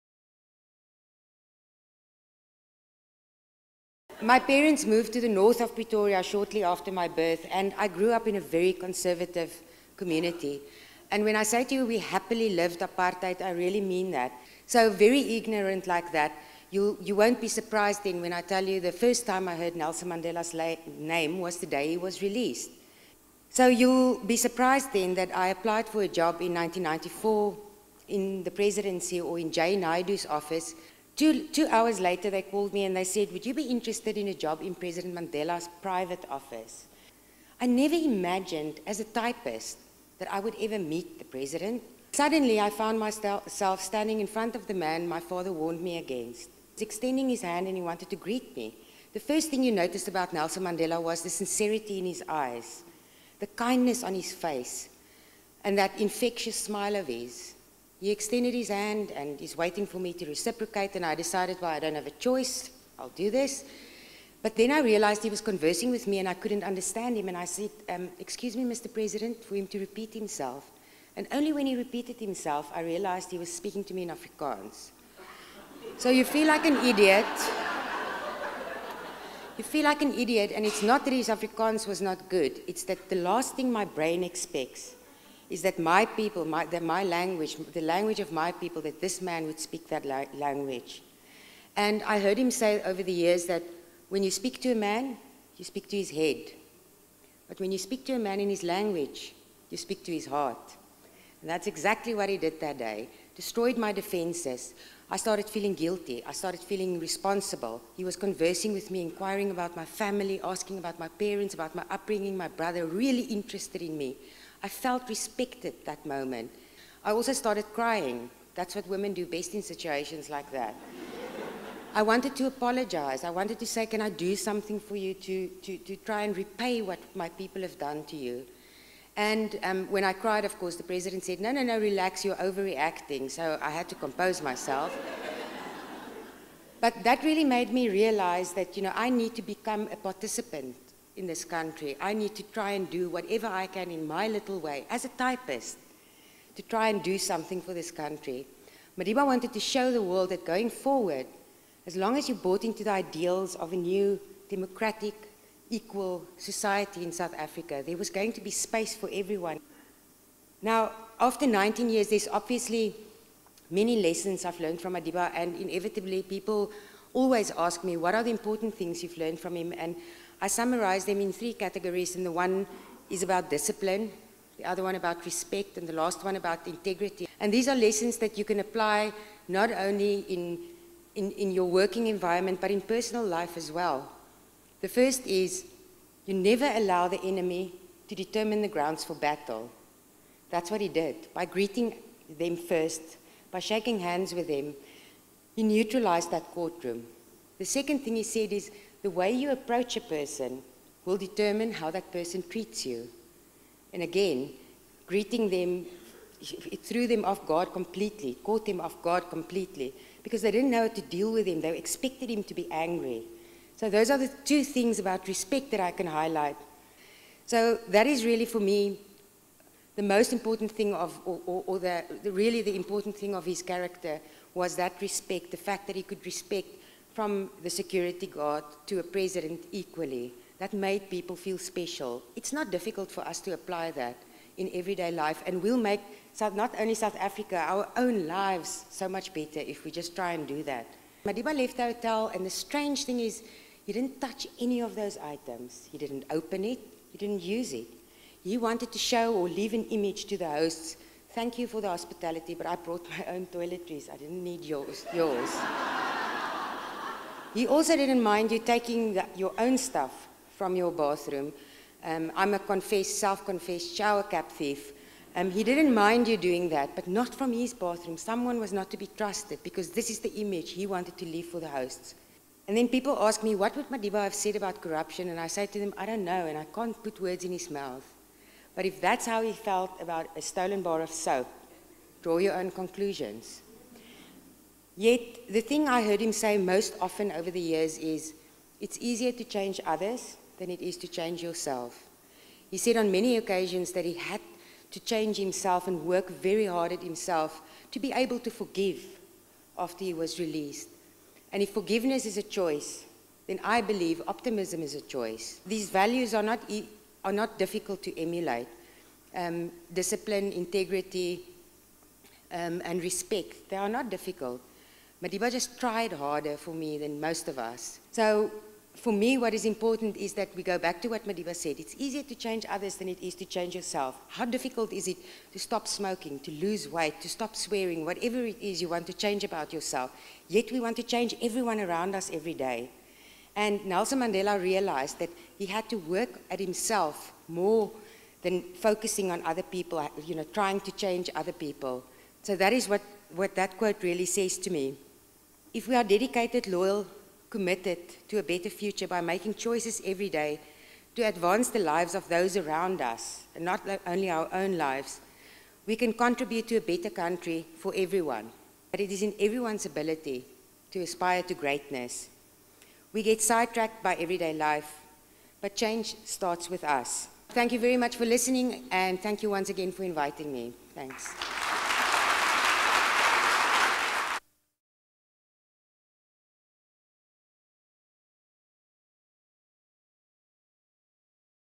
SAACI 2015: Highlights from Zelda la Grange's keynote address
This video clip contains highlights from Zelda la Grange's keynote address at the SAACI 2015 Congress that took place at The Westin Hotel in Cape Town from 2 - 4 August 2015. La Grange was Nelson Mandela's personal assistant for many years, and is the author of Good Morning Mr Mandela.